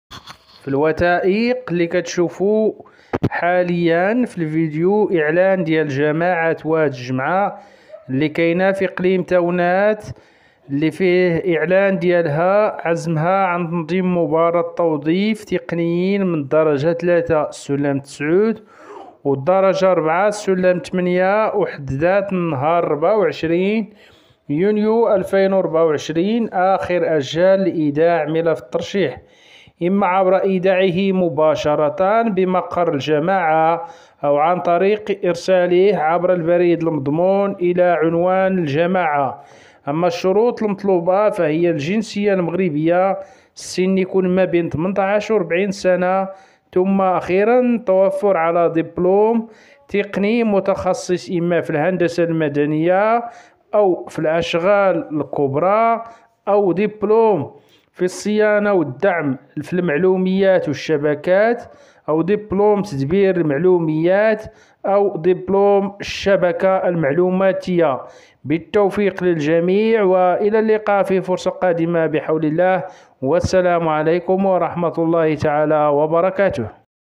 التوظيف في الجماعات الترابية: توظيف مجموعة من التقنيين في جماعة بإقليم تاونات الشروط وطريقة الترشيح
التوظيف في الجماعات الترابية: توظيف مجموعة من التقنيين في جماعة بإقليم تاونات الشروط وطريقة الترشيح#الرئيس#كونكور_جديد#مباراة#الأجر#المملكة_المغربية